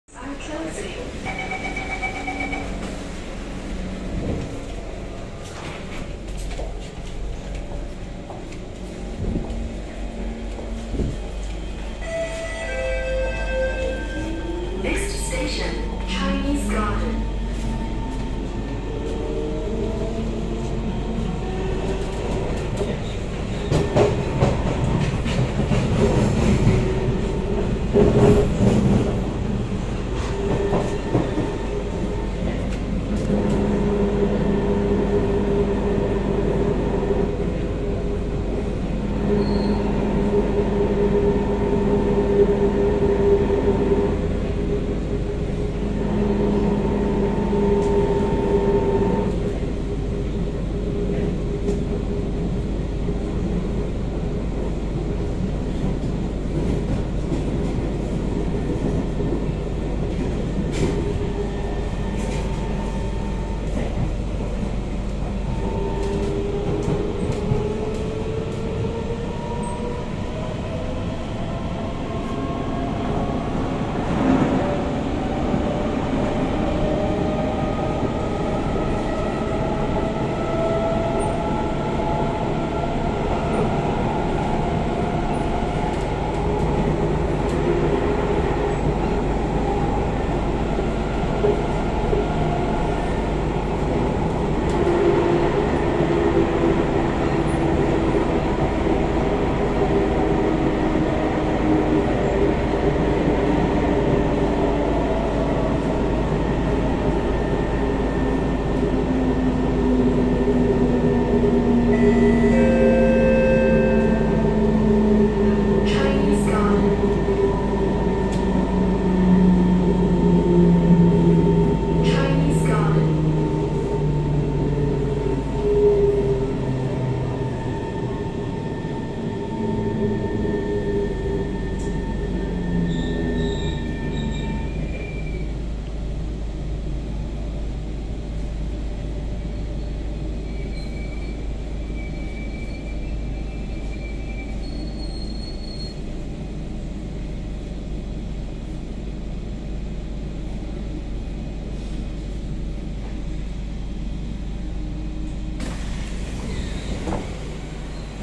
[British Thrasher EWL] SMRT KHI C151 [005/006] (Jurong East → Chinese Garden)
Date filmed: 21 October 2022
Time of recording: 0906hrs
Towards: (2) Tuas Link
Train Model: Kawasaki Heavy Industries C151
Train Run: 318
Depot of Origin: Tuas West Depot
Final Destination Depot: Tuas West Depot
Carriage filmed at: 2006 A4/B4
AM/PM/Full Day: TWD AM Peak Service

The British Thrasher is back running over on EWL, after a number of months spent on NSL. One of the rides which returns back to TWD after AM Peak, and will await for PM Peak Deployment. Strong Motor can be heard on car 2006 A4.